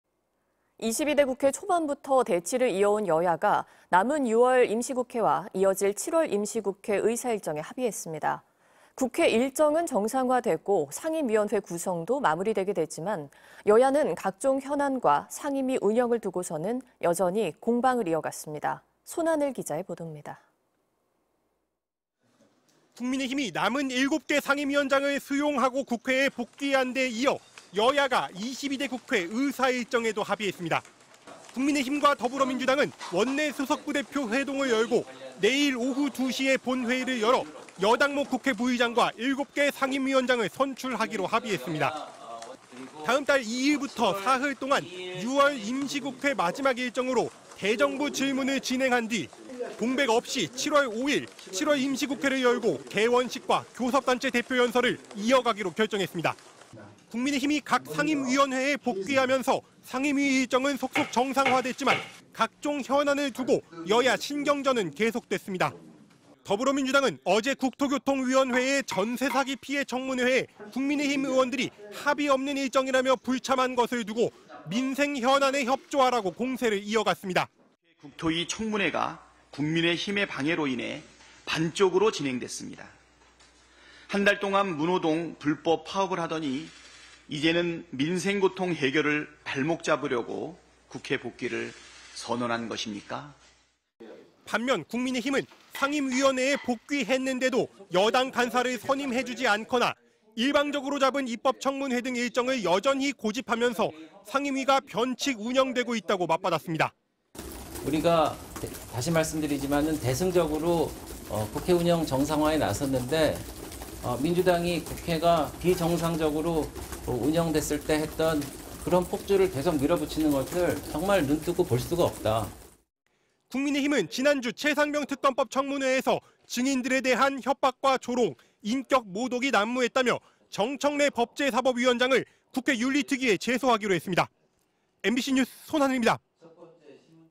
국회 의사일정 합의‥내일 원 구성 마무리 (2024.06.26/12MBC뉴스)
22대 국회 초반부터 대치를 이어온 여야가, 남은 6월 임시국회와 이어질 7월 임시국회 의사일정에 합의했습니다.

국회 일정은 정상화되고 상임위원회 구성도 마무리되게 됐지만, 여야는 각종 현안과 상임위 운영을 두고서는 여전히 공방을 이어갔습니다.